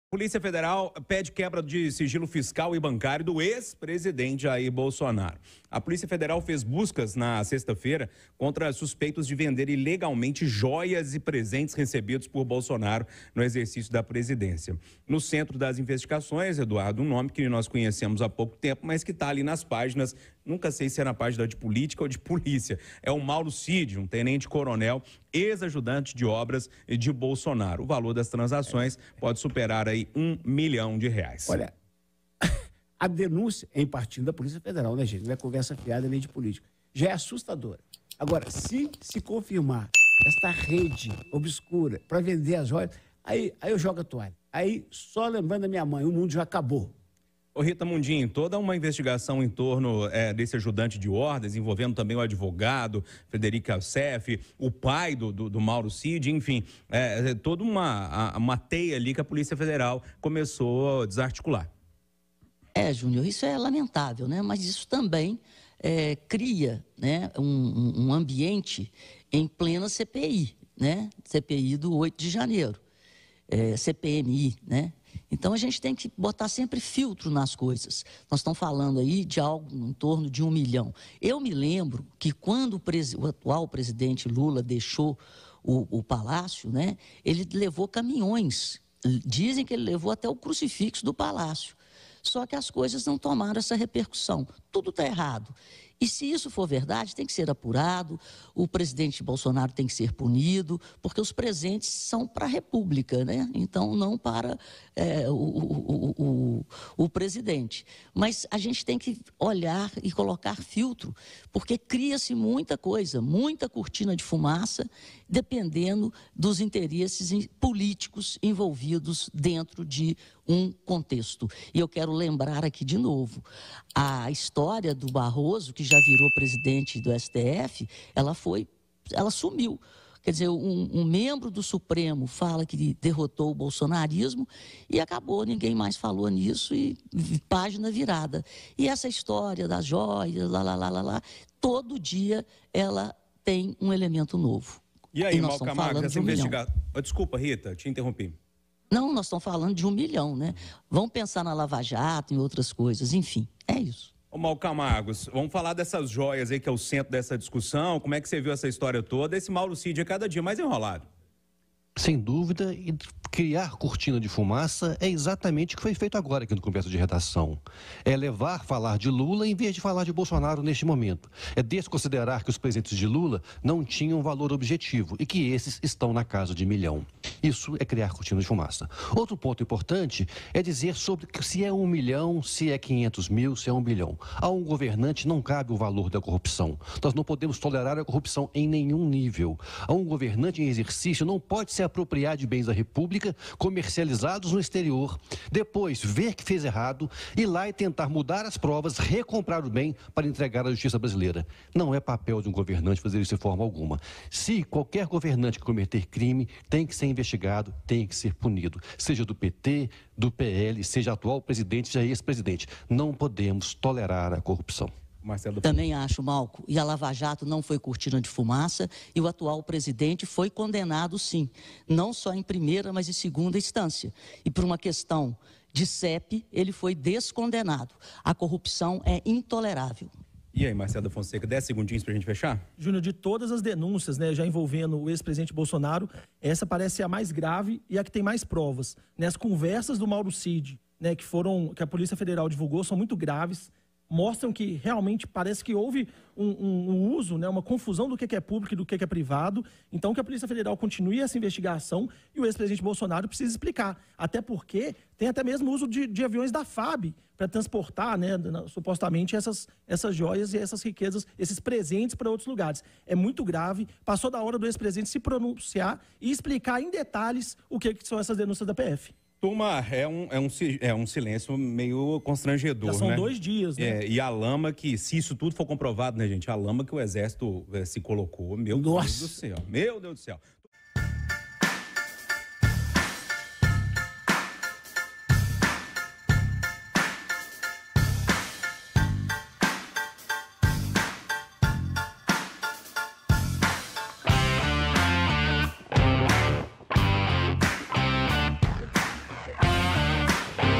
JUSTIÇA PEDE FIM DE SIGILO BANCÁRIO DE JAIR BOLSONARO | CONVERSA DE REDAÇÃO
A Polícia Federal pediu a quebra dos sigilos fiscal e bancário da ex-primeira-dama Michelle Bolsonaro para investigar se o ex-presidente Jair Bolsonaro utilizou contas da esposa para fazer transações de valores de presentes do Estado vendidos ilegalmente. As informações foram divulgadas pela CNN na noite deste sábado (12).